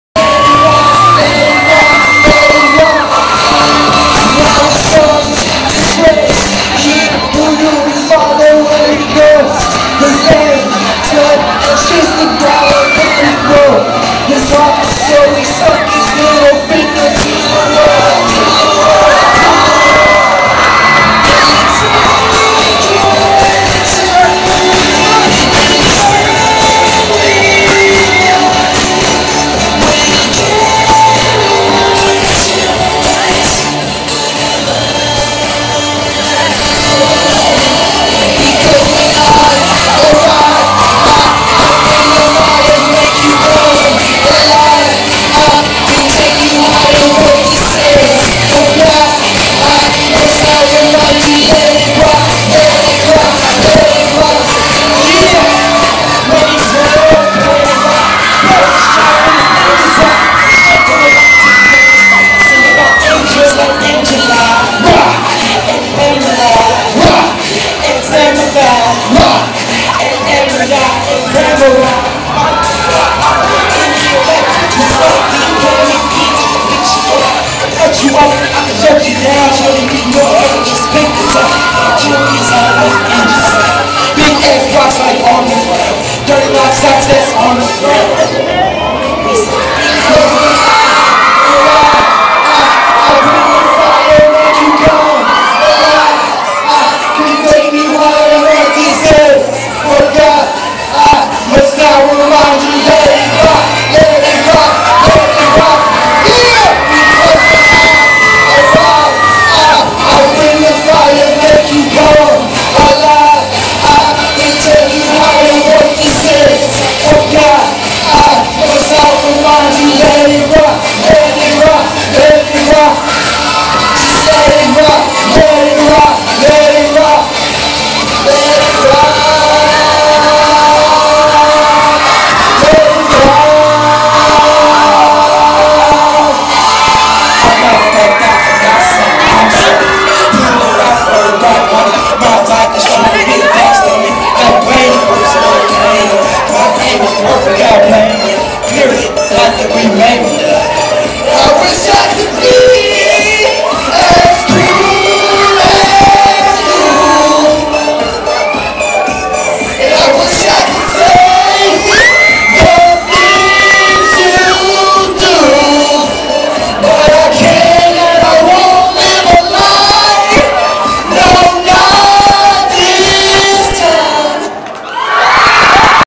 the kennedy pep rally
video of the pep rally (real shitty qulity) music from ben&johnny